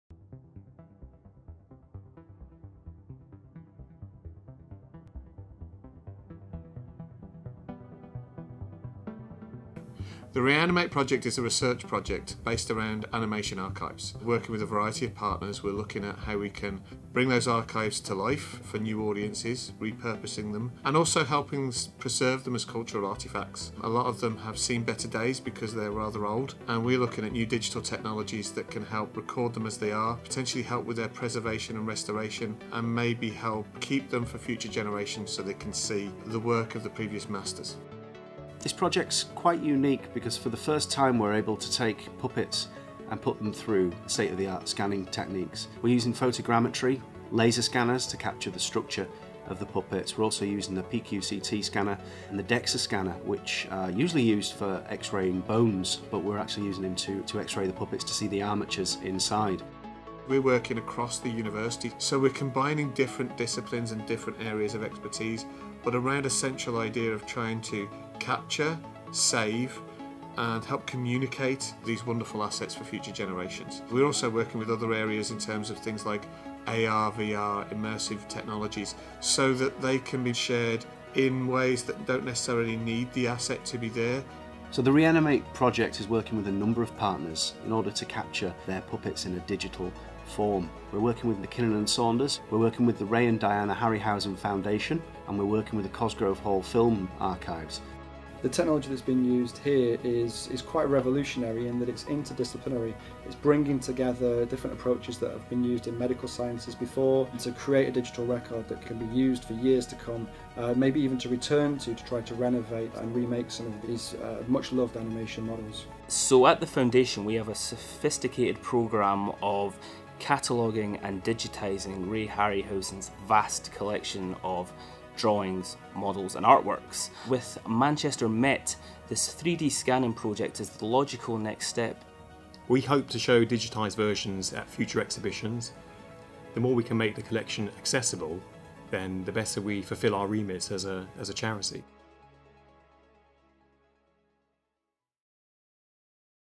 ReAnimate Project: Bringing Stop Motion Characters Back to Life
Stop motion animation is a painstaking and powerful visual effect – but what happens to the models and memories after filming?

In our latest issue of Met Magazine - Manchester Metropolitan University's dedicated bi-annual publication - we take a look at the ReAnimate Project, which is bringing classic characters back to life for audiences old and new as part of a collaboration between the University, The Ray and Diana Harryhausen Foundation, the Waterside Arts Centre and Altrincham-based animation producers, McKinnon and Saunders.

Read our Met Magazine feature 'Back to Life' about the project